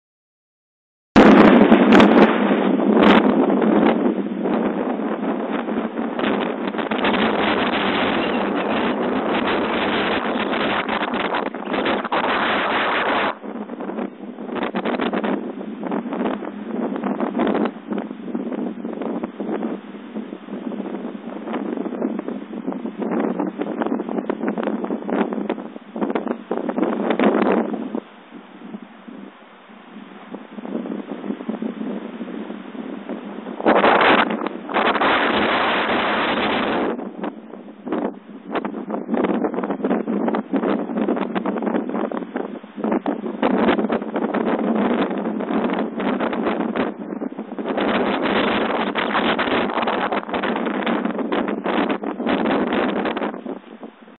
Windy windy windy
Ridiculously windy at my house.